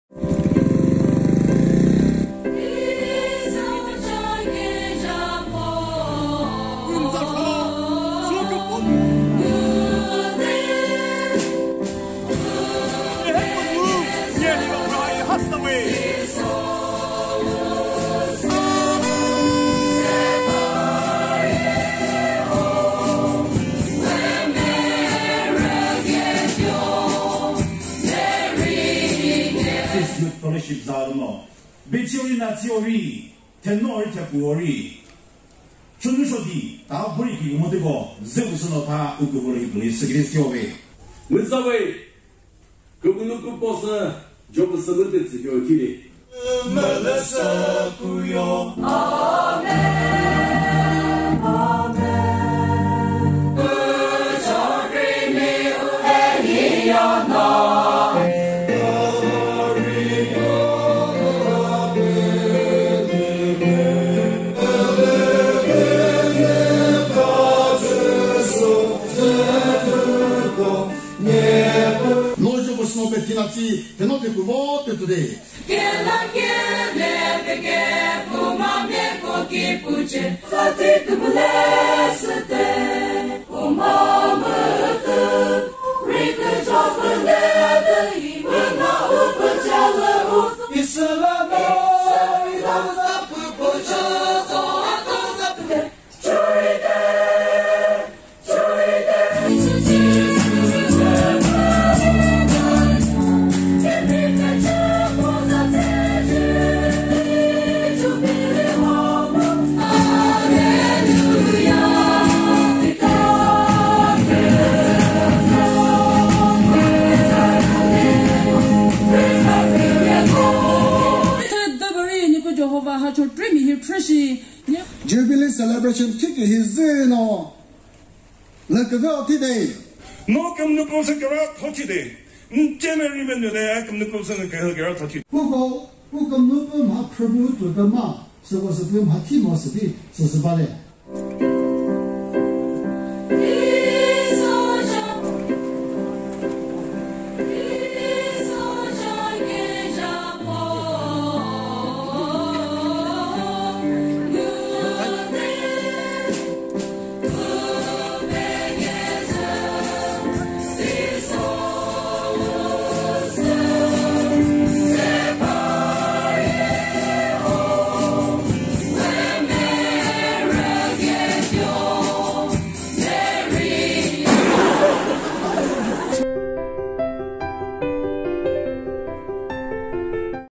Chokri Area Baptist Youth Fellowship, 50th Year celebration
Chokri area Baptist youth fellowship 50th year celebration at Thenyizu Baptist Church.

This is just a Trailer video, Coming soon full video.

Speakers:
1. Rev. P.  Bonny Resu
Lead Pastor, Chakhesang Baptist Church, Dimapur
2. Rev. Dr. Suluveyi Rhakho
Director, Harvest ministries international
3. Rev. Dr. Chekroveyi cho-o
Professor of Applied Theology, Oriental Theological Seminary, Bade, Dimapur

Resource person:
Mr. Colo Mero, Director life ministries.